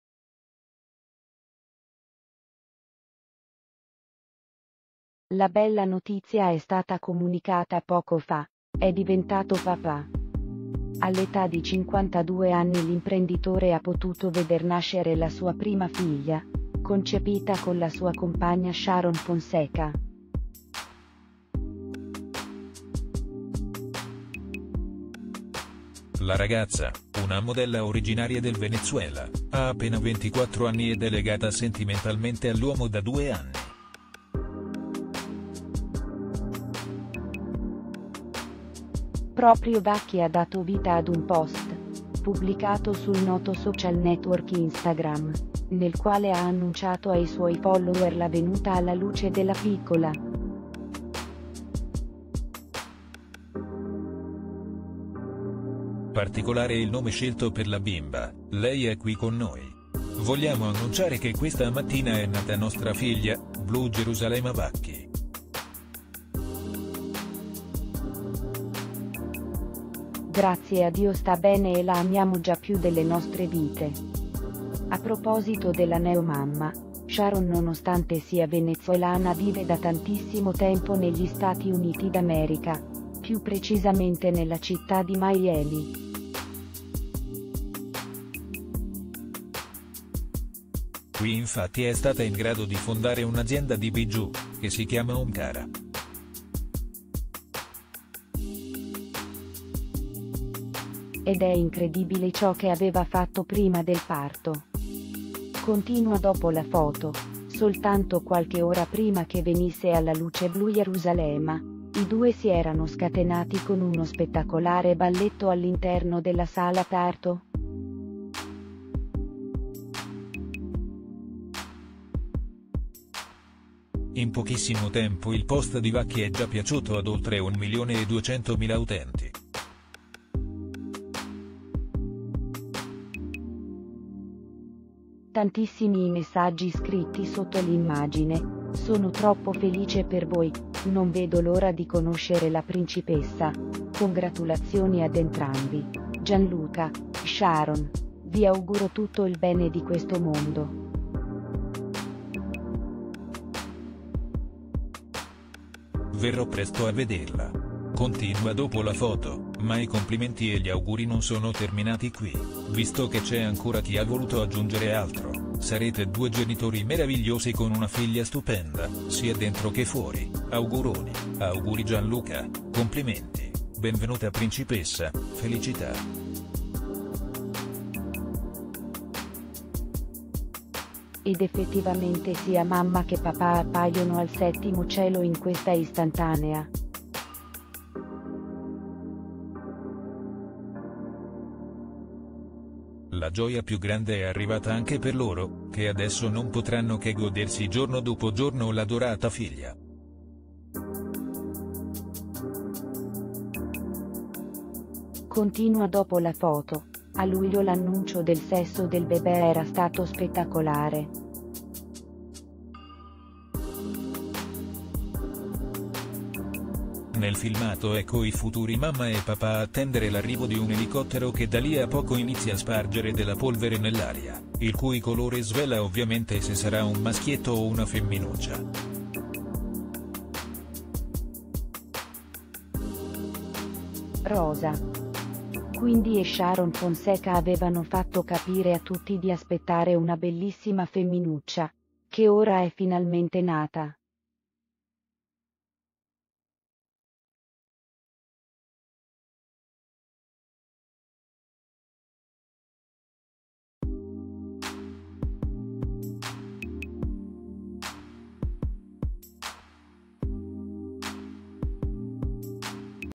“È nata!”. Gianluca Vacchi papà per la prima volta
La bella notizia è stata comunicata poco fa: è diventato papà. All’età di 52 anni l’imprenditore ha potuto veder nascere la sua prima figlia, concepita con la sua compagna Sharon Fonseca. La ragazza, una modella originaria del Venezuela, ha appena 24 anni ed è legata sentimentalmente all’uomo da 2 anni. Proprio Vacchi ha dato vita ad un post, pubblicato sul noto social network Instagram, nel quale ha annunciato ai suoi follower la venuta alla luce della piccola.  Particolare il nome scelto per la bimba: “Lei è qui con noi. Vogliamo annunciare che questa mattina è nata nostra figlia, Blu Jerusalema Vacchi. Grazie a Dio sta bene e la amiamo già più delle nostre vite”. A proposito della neo mamma, Sharon nonostante sia venezuelana vive da tantissimo tempo negli Stati Uniti d’America, più precisamente nella città di Miami. Qui infatti è stata in grado di fondare un’azienda di bijoux, che si chiama ‘Omkara’. Ed è incredibile ciò che aveva fatto prima del parto. (Continua dopo la foto)        Soltanto qualche ora prima che venisse alla luce Blu Jerusalema, i due si erano scatenati con uno spettacolare balletto all’interno della sala parto. In pochissimo tempo il post di Vacchi è già piaciuto ad oltre un milione e 200 mila utenti. Tantissimi i messaggi scritti sotto l’immagine: “Sono troppo felice per voi, non vedo l’ora di conoscere la principessa”, “Congratulazioni ad entrambi”, “Gianluca, Sharon, vi auguro tutto il bene di questo mondo. Verrò presto a vederla”. (Continua dopo la foto)       Ma i complimenti e gli auguri non sono terminati qui, visto che c’è ancora chi ha voluto aggiungere altro: “Sarete due genitori meravigliosi con una figlia stupenda, sia dentro che fuori”, “Auguroni”, “Auguri Gianluca”, “Complimenti, benvenuta principessa”, “Felicità”. Ed effettivamente sia mamma che papà appaiono al settimo cielo in questa istantanea. La gioia più grande è arrivata anche per loro, che adesso non potranno che godersi giorno dopo giorno l’adorata figlia. (Continua dopo la foto)         A luglio l’annuncio del sesso del bebè era stato spettacolare. Nel filmato ecco i futuri mamma e papà attendere l’arrivo di un elicottero che dà lì a poco inizia spargere della polvere nell’aria, il cui colore svela ovviamente se sarà un maschietto o una femminuccia. Rosa! Quindi e Sharon Fonseca avevano fatto capire a tutti di aspettare una bellissima femminuccia, che ora è finalmente nata.